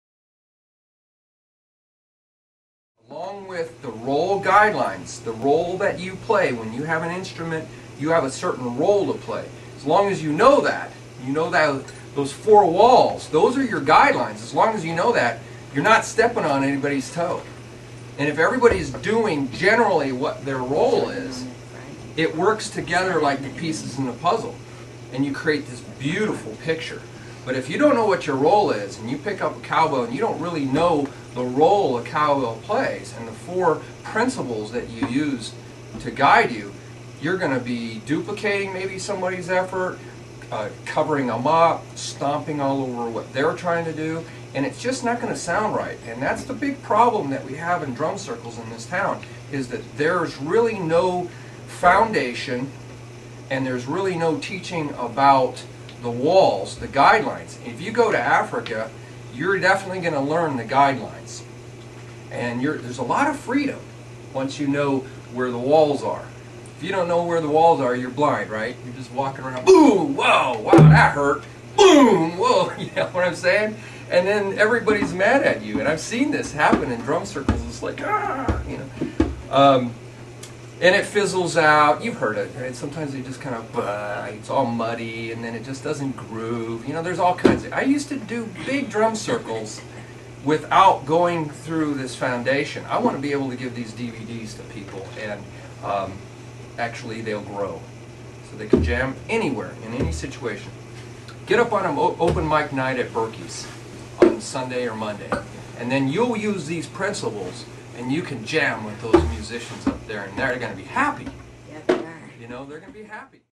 CoreBeat - FREE Intro - Skill-Set and Role-Guideline ONLINE Videos and DVDs
I have used ALL types of drums and percussion to simply illustrate the CoreBeat System's principles and methods that will help ANYBODY learn ALL types of rhythm and music FAST while having a BLAST!

◦Play ANY instrument, drum, sing, or dance more rhythmically and musically . . .
◦Starting at ANY skill level - beginning, intermediate, or advanced . . .
◦Using my simple guidelines to quickly go to the next level - even after 1 session . . .
◦So you can jam "on the spot" with other people . . .
◦To ANY music styles . . .
◦And have a BLAST doing it!

- Click on the CoreBeat tab!